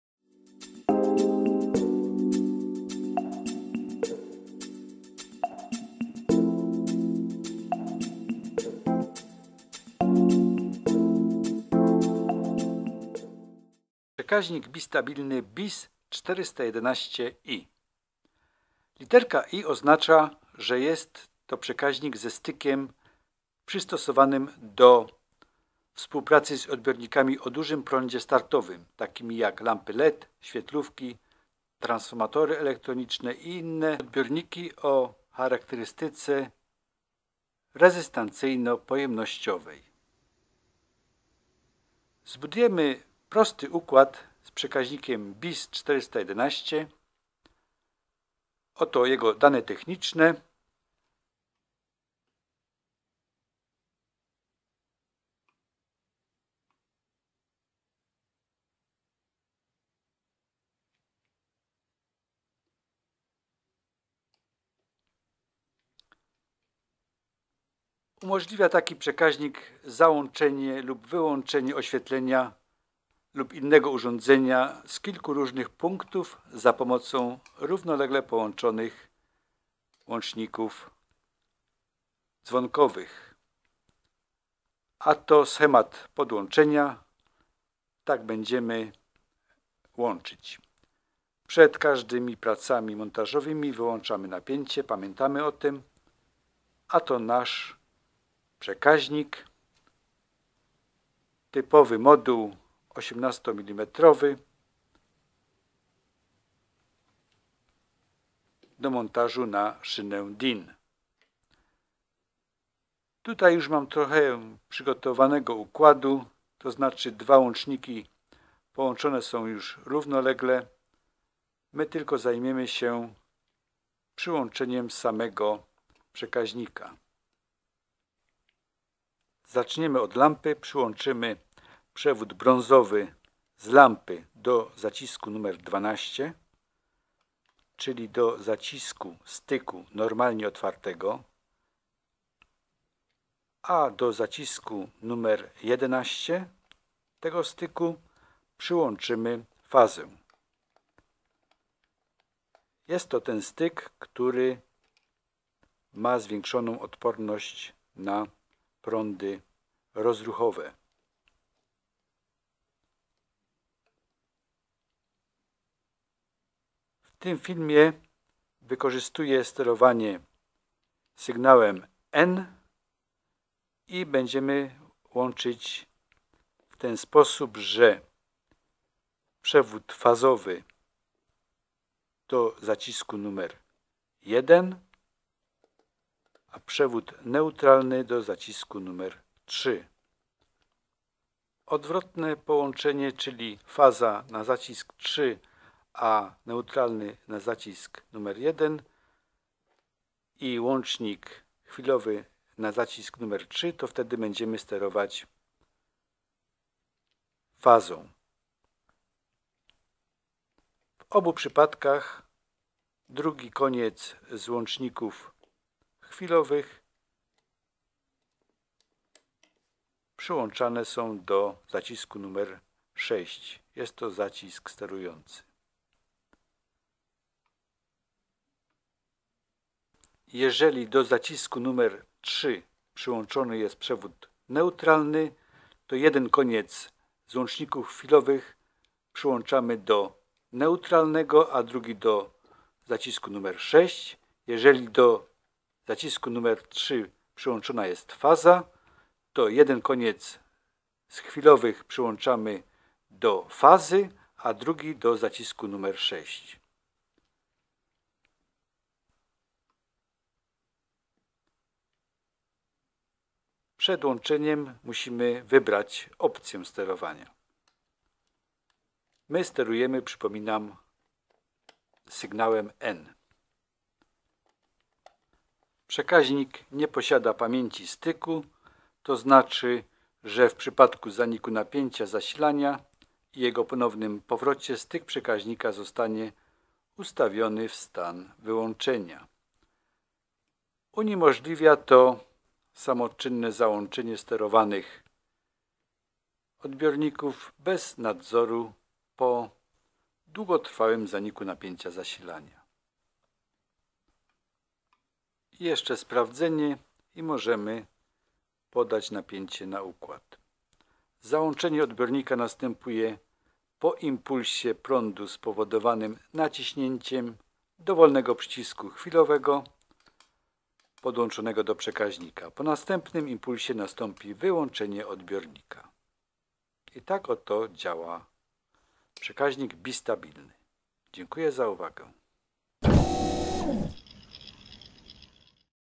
Poradnik praktyka elektryka - BIS 411LED - przekaźnik bistabilny do oświetlenia LED -
Dlaczego warto stosować przekaźniki bistabilne ze stykiem inrush do nowoczesnego oświetlenia. Styk inrush charakteryzuje się wysoką wytrzymałością na piki prądowe przy zapłonie lamp np. LED.
W przypadku BIS-411i wynosi 160A/20ms.